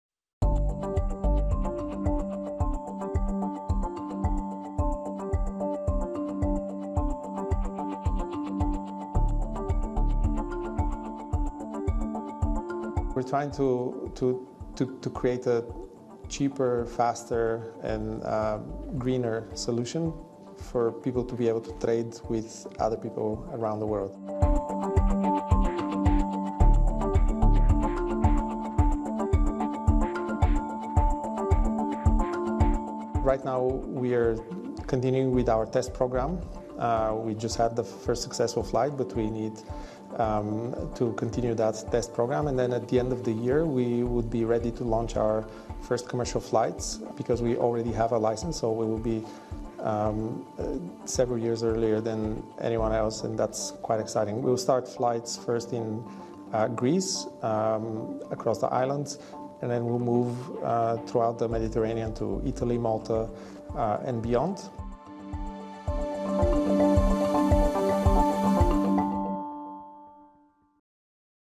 This Bulgarian start-up is reimagining how we move goods by air with its cargo drones
Bulgarian firm Dronamics has just completed test flights of their full-size cargo drone prototype with hopes to launch commercial operations later this year.